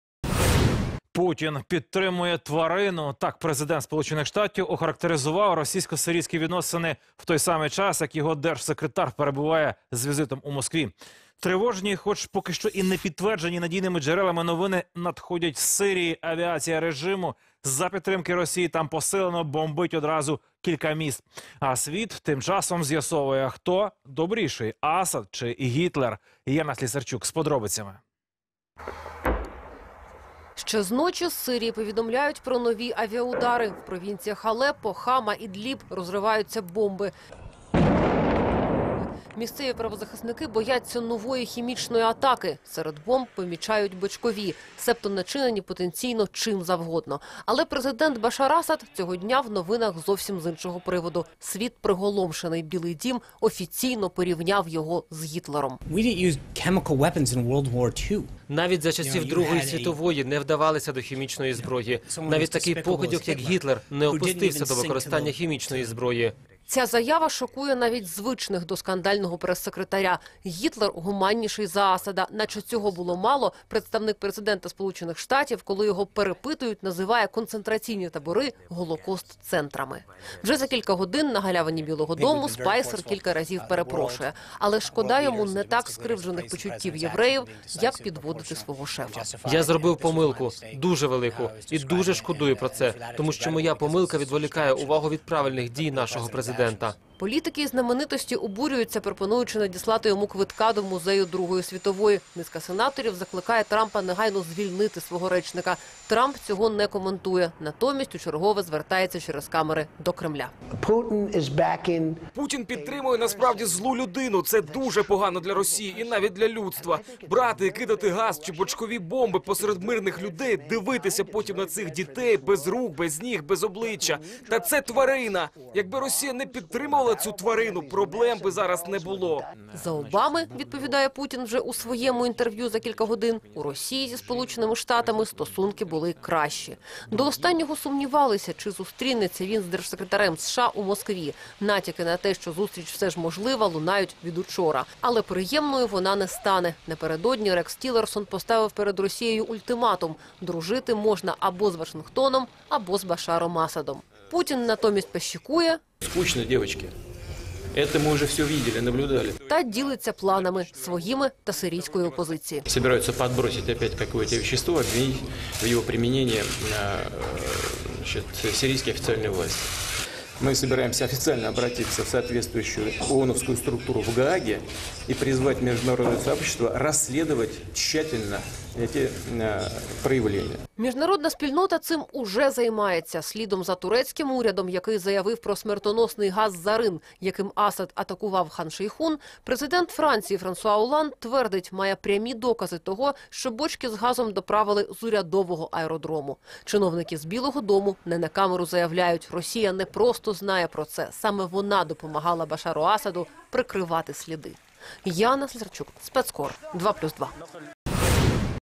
Білий дім порівняв президента Сирії з Гітлером
Світ приголомшений: прес-секретар Білого дому Шон Спайсер порівняв президента Сирії Башара Асада з Адольфом Гітлером. Асад використовував отруйний газ проти власного народу. Низка сенаторів закликає Трампа негайно звільнити свого речника.